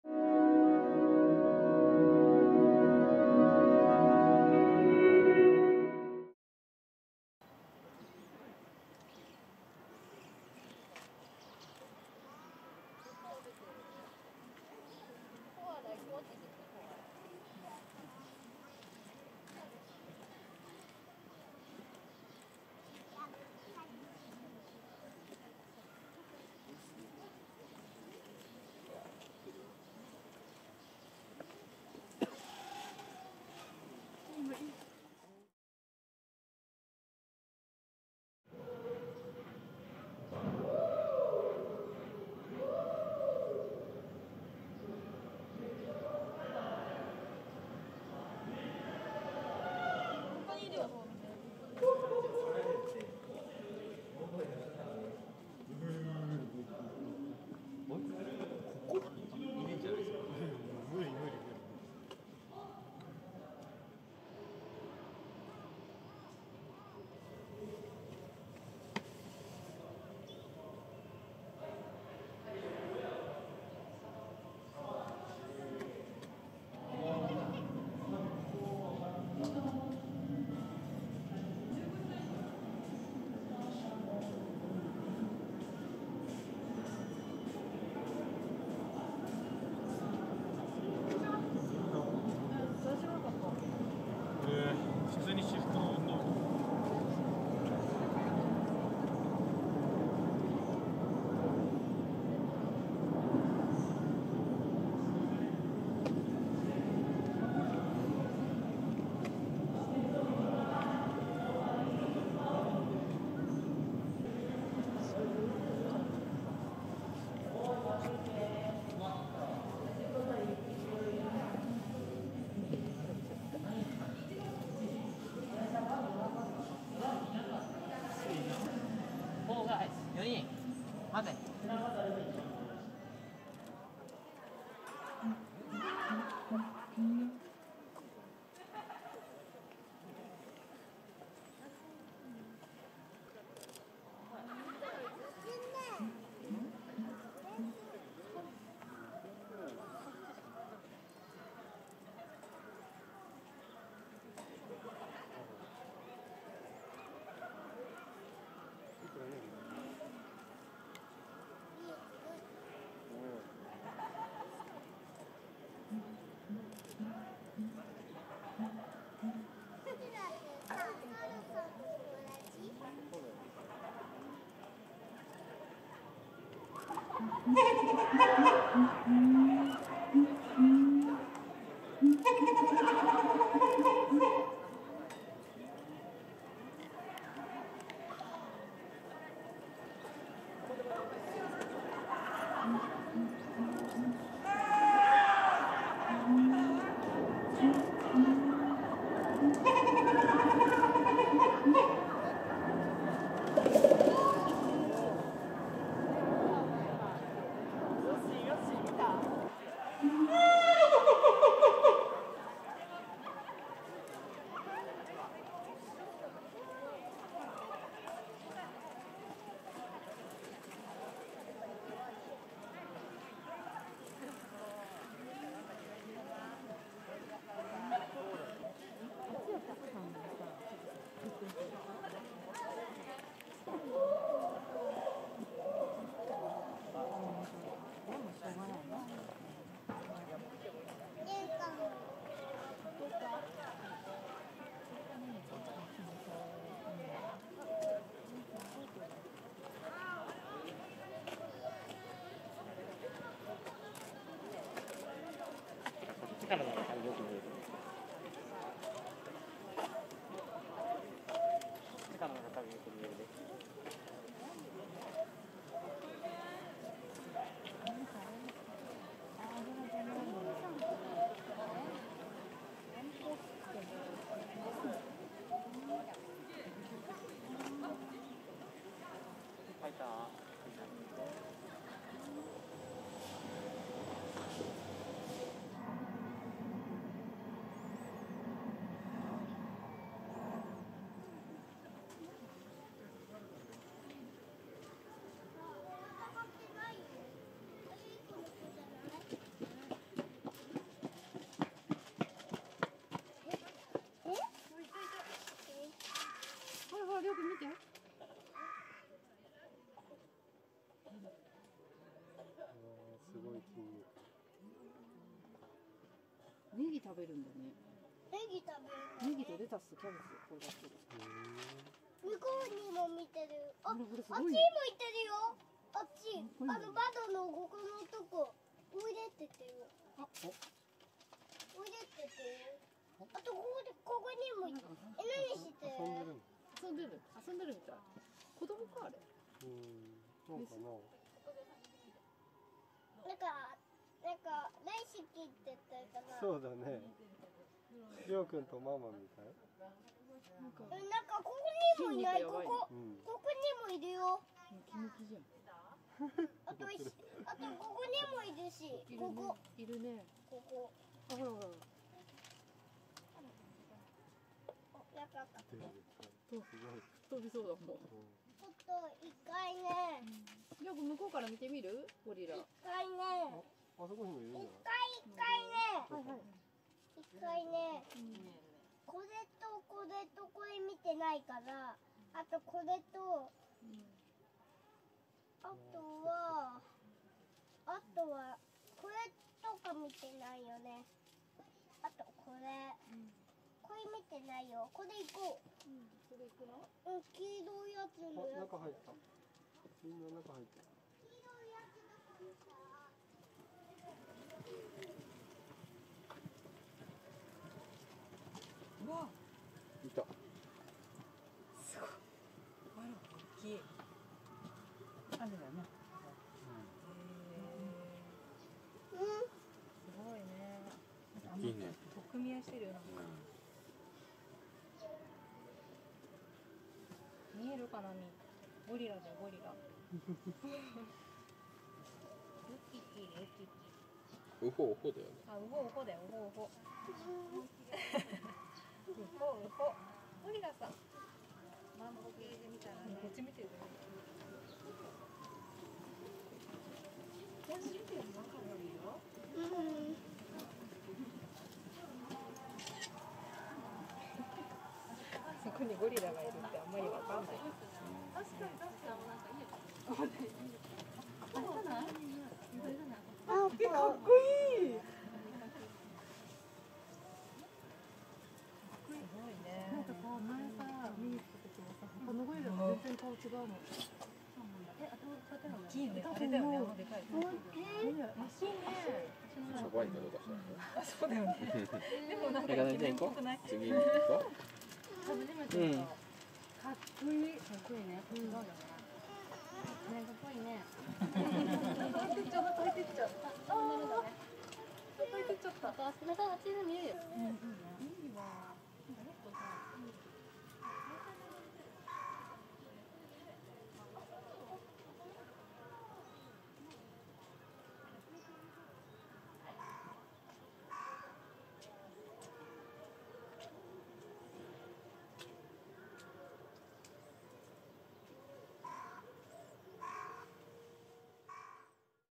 Silverback Gorilla Trying To Mend Fences With His Son | The Shabani Family
It has been about three weeks since the big fight between Shabani and his son Kiyomasa.
The gorillas eat well and look very healthy.
However, it was difficult to see Shabani and Kiyomasa playing together, but today they were playing wrestling together.
They are getting back to their usual Hilarious Shabani family.

Time Stamps
00:00 Shabani and Kiyomasa playing together
00:07 Kiyomasa
00:35 Gorillas on March 1, 2023
02:13 Shabani teasing Kiyomasa
04:42 Shabani and the boy
04:51 Shabani and Kiyomasa